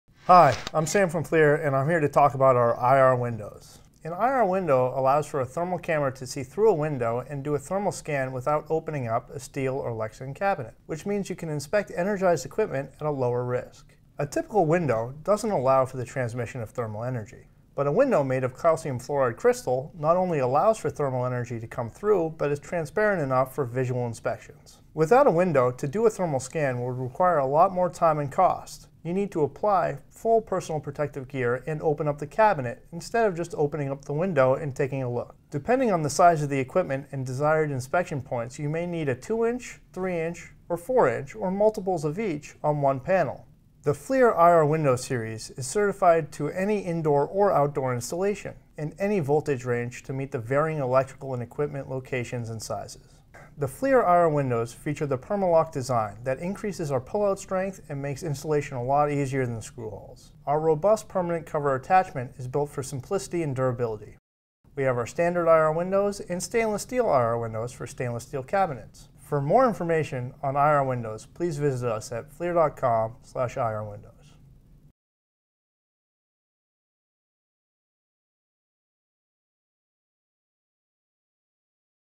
Introducing FLIR IR Windows for Infrared Inspection
Anywhere from 5 to 10 arc flash accidents occur every day in the U.S., resulting in severe burn injuries that affect over 2,000 workers annually. The costs in lives, livelihoods, and liability claims can be enormous. Fortunately, new FLIR IR Windows now offer a solution to help reduce exposure so you can perform scans safely, more efficiently, and confidently that comply with NFPA 70E requirements.